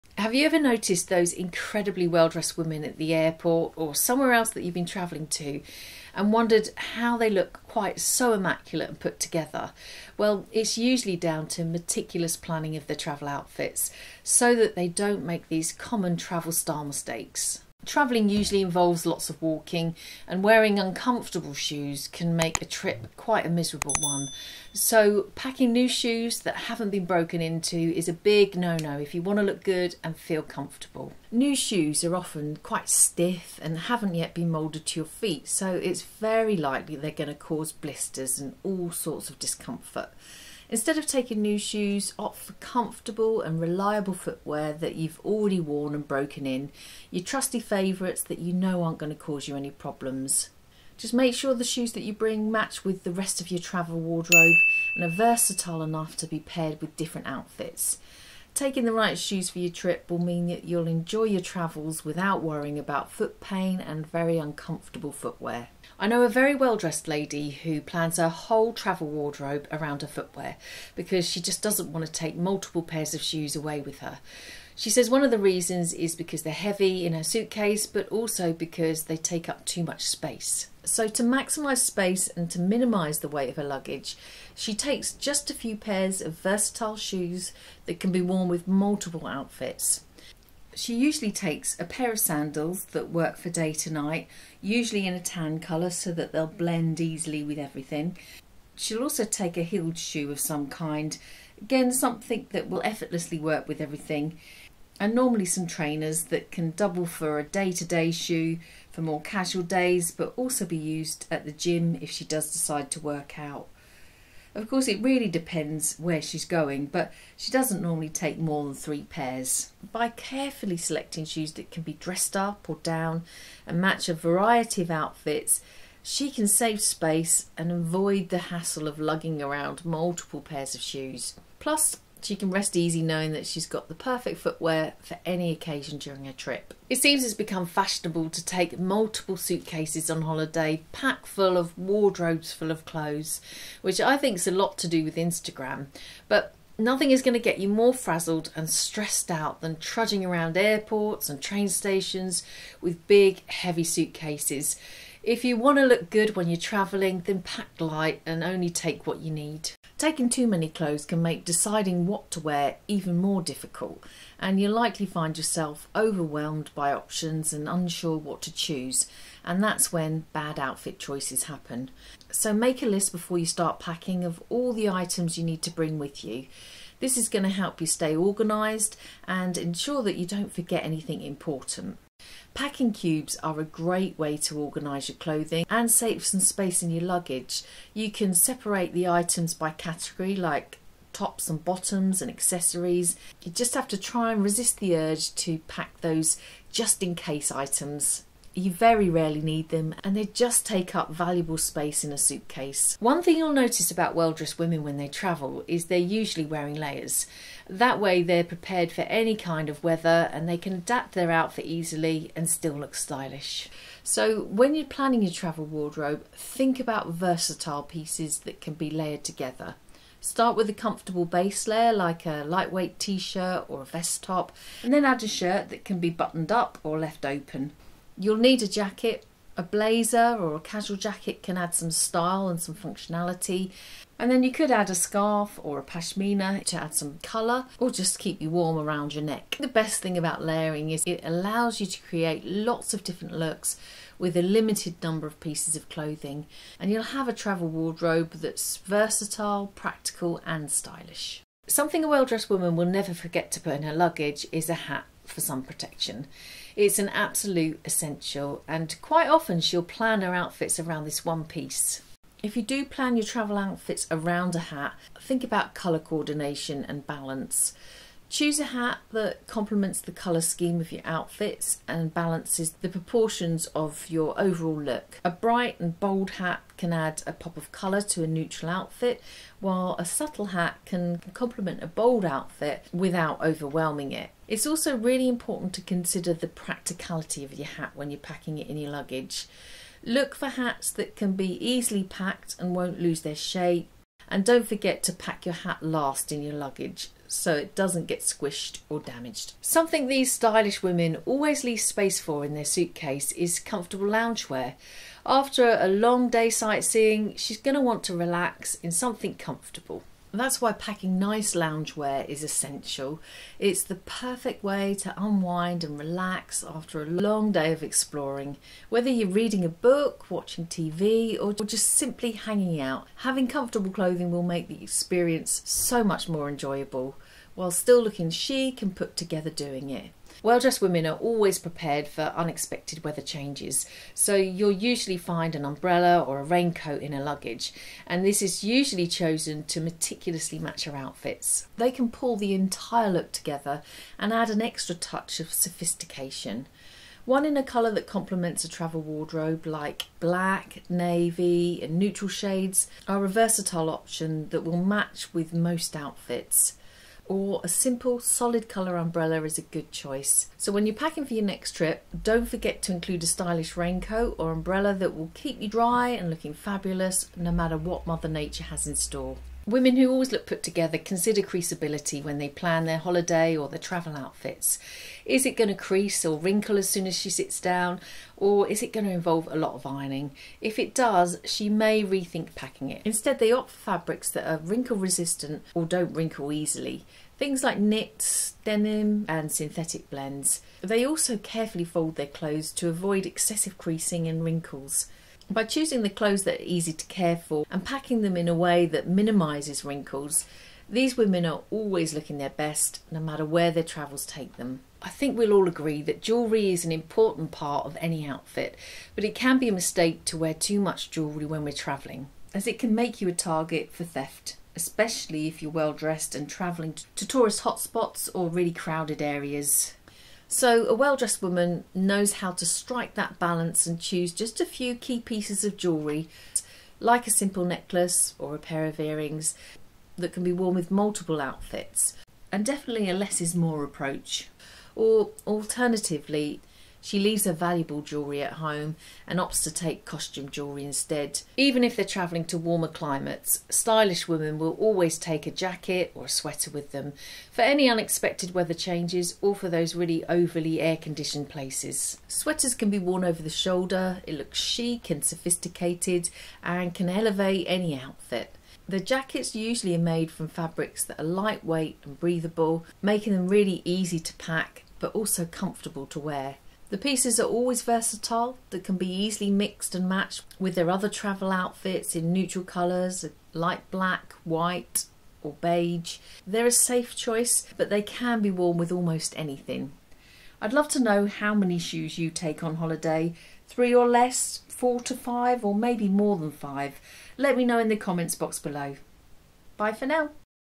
10 Style mistakes a WELL-DRESSED woman NEVER makes when travelling
Travelling can be stressful, and it's not just because of the logistics and planning. It's also about what to pack, what to wear, and how to make sure you're comfortable while on the go. In this video, I'll share some things to make your travel experience as well-dressed as possible.

These will help you save time putting together outfits, save you dressing stress and maximise your wardrobe.

More coming soon, to be the first to be notified sign up below 💌

⬆⬆⬆ Above is a list of the best white shirts (for all budgets)